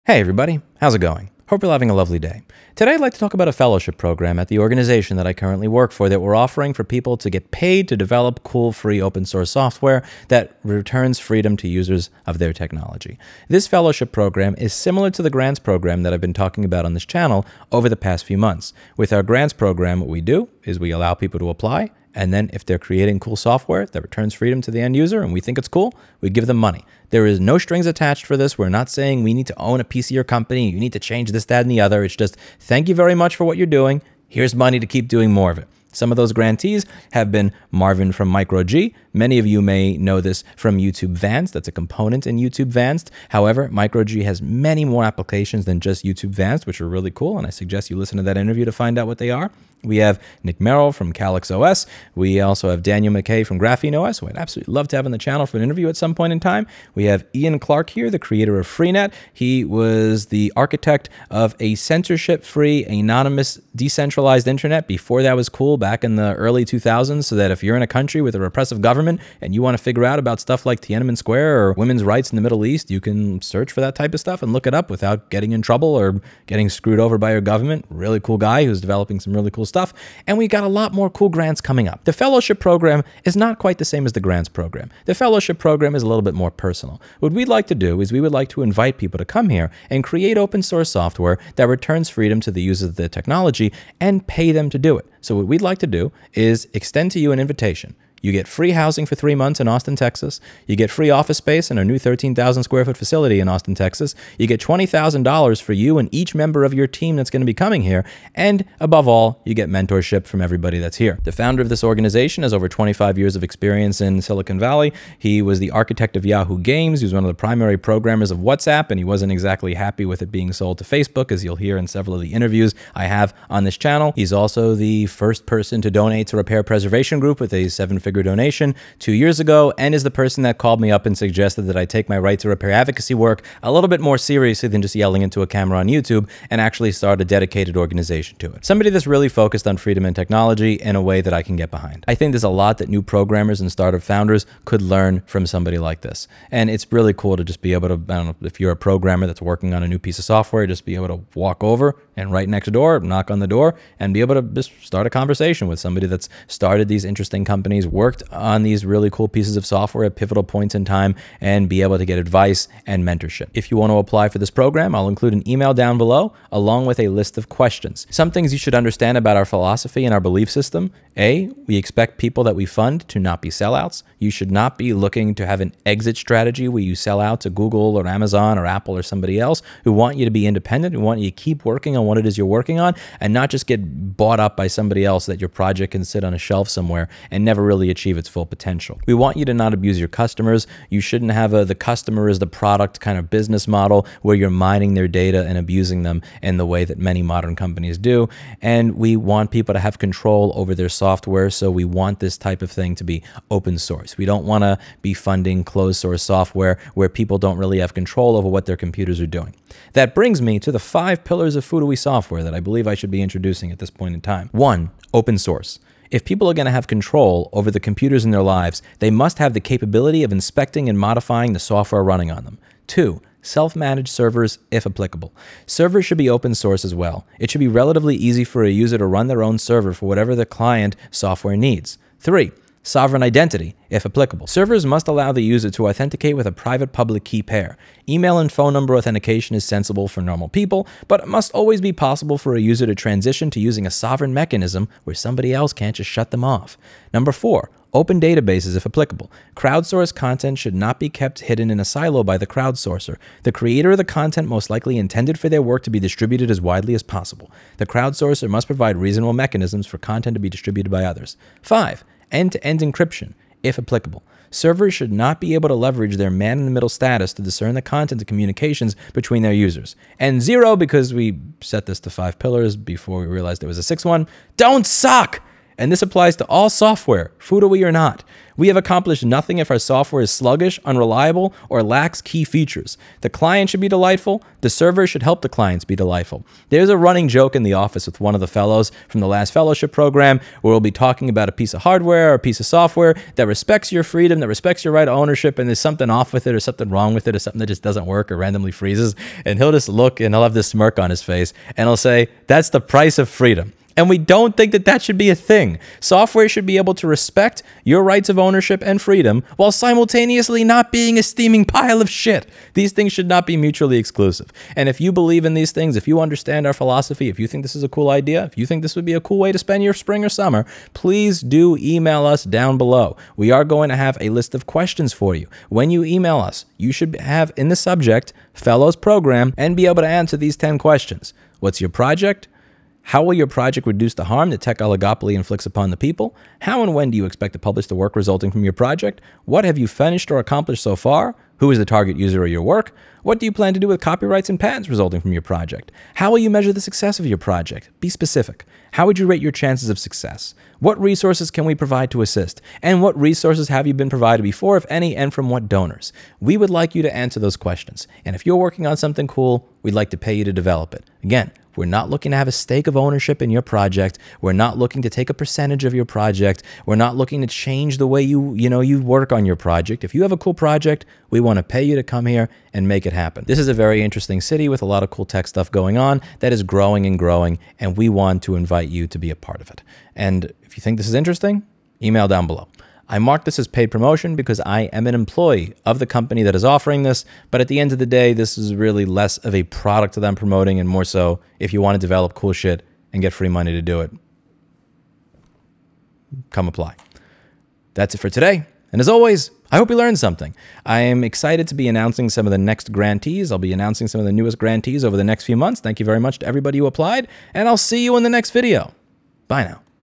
Paid fellowship program to create open source software
If selected, each member of your team will receive at least $20,000 of funding, airfare to Austin, free housing for three months, incubator space, equipment, mentorship, and VIP invites to a full slate of events.

Applicants are asked to email a CV and 500–1000 word description of their project to . Shortlisted candidates will be invited to interview with FUTO staff at our Austin offices or, if the candidate is outside Central Texas, via video chat. Please apply by 2023 Jan 1.

Here are some questions we’ll be asking to judge applicants:

1.    What is your project? Provide a link to more detailed information if it exists.
2.    How will your project reduce the harm that the Tech Oligopoly inflicts upon the people?
3.    How and when do you expect to publish the work resulting from your project?
4.    What have you finished or accomplished so far?
5.    Who is the target user of your work?
6.    What do you plan to do with copyrights and patents resulting from your project?
7.    How will you measure the success of your project? Please be as specific as possible.
8.    How would you rate your chances of success?
9.    What resources can the FUTO organization provide to assist?
10.    What resources have you been provided before if any, and from what donors?

Best of luck to all applicants!